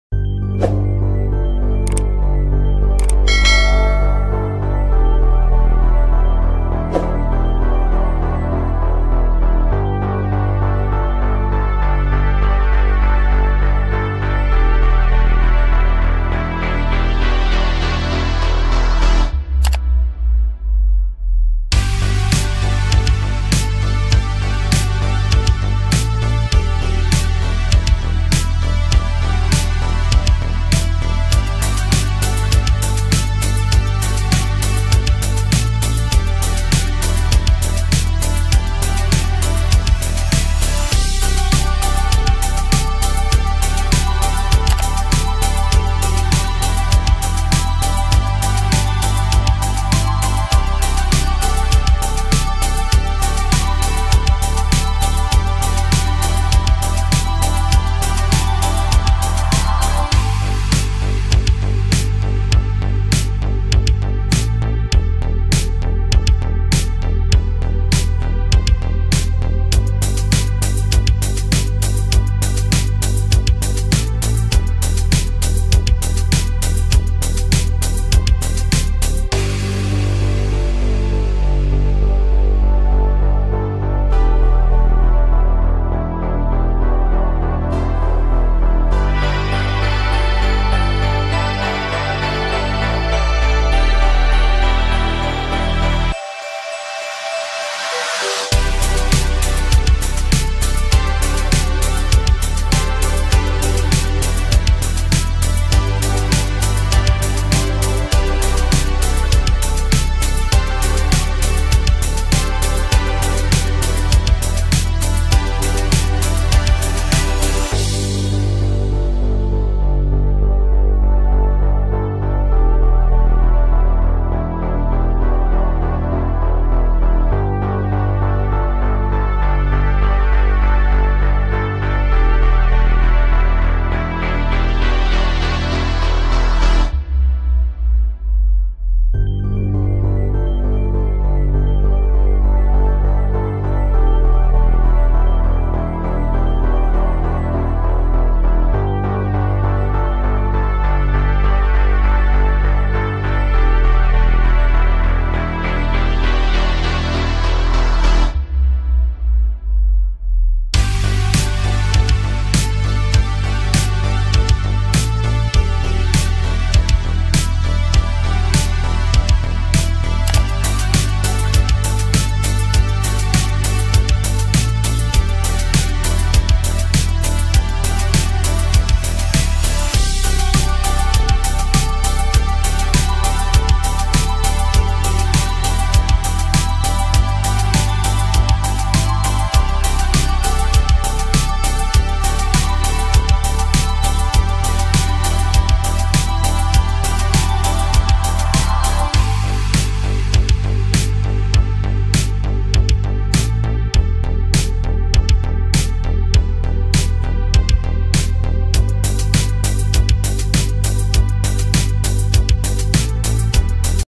Important Physics question for odisha police constable communication/communication constable police
Day 14 communication constable Physics Question by srikrishna Darshana Odia
2021 all govt exam Physics Question like railway,ssc,osssc,ossc odisha
Odisha previous year govt exam questions